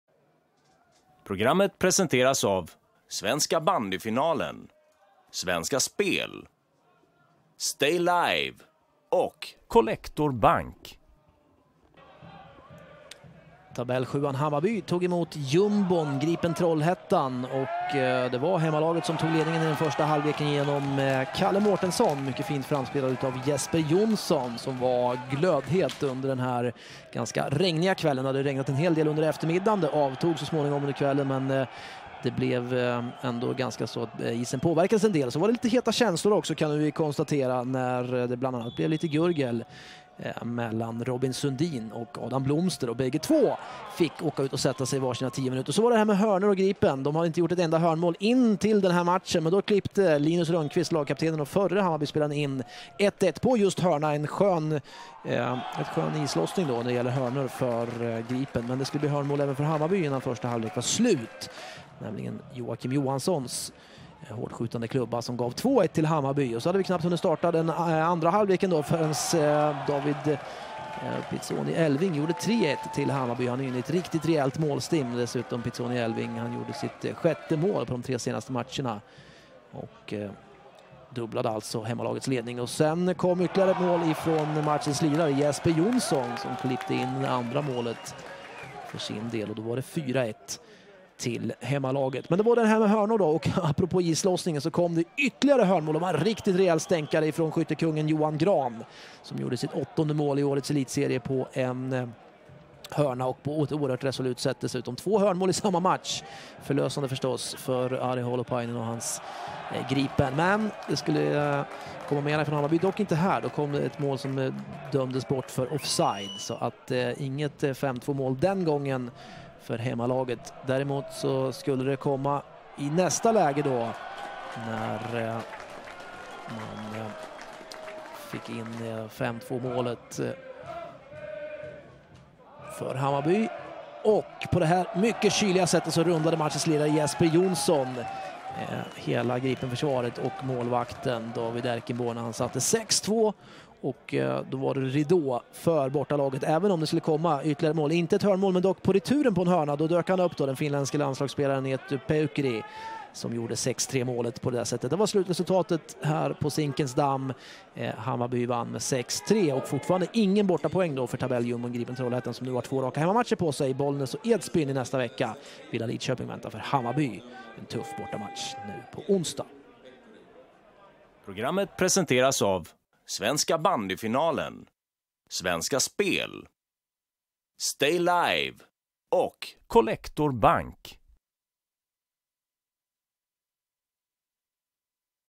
18.11.2016. Элитсерия. "Хаммарбю" - "Грипен" - 6:3
"Хаммарбю" - "Грипен" - 6:3 (2:1). Голы: К.Мортенссон (9), Й.Юханссон (42), Пиццони-Эльфинг (51), Й.Юнссон (53, 72, 79) - Рённквист (18), Гран (59), Э.Пеухкури (83).